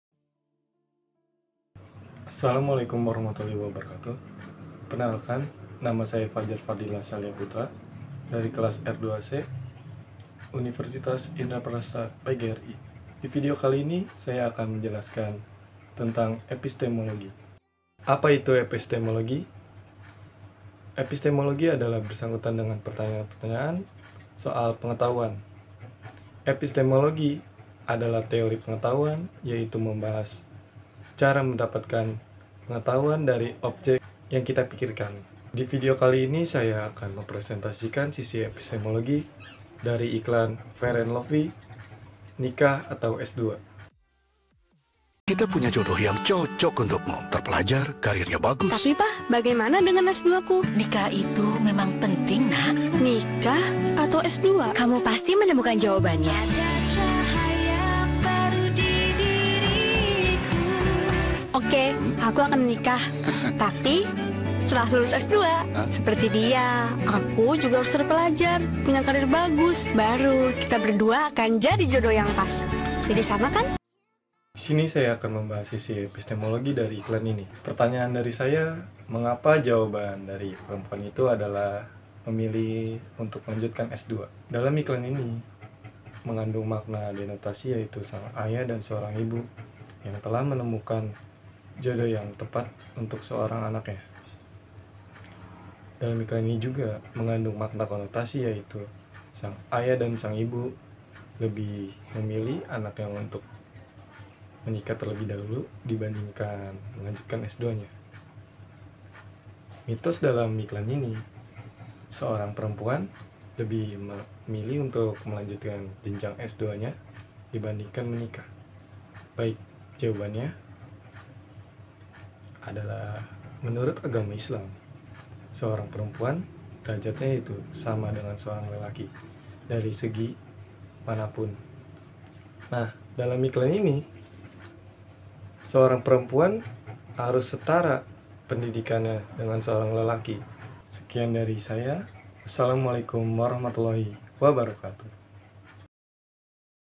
Fajar Fadlilah Salya Putra (Epistemologi Iklan Fair & Lovely)
Penjelasan Epistemologi Fair & Lovely
Nama : Fajar Fadlilah Salya Putra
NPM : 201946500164
Kelas : R2C
Diunggah untuk memenuhi tugas akhir semester genap mata kuliah filsafat ilmu
Dosen : Nurul Azmy, M.Hum.
Program Studi : S1 Desain Komunikasi Visual
Universitas Indraprasta PGRI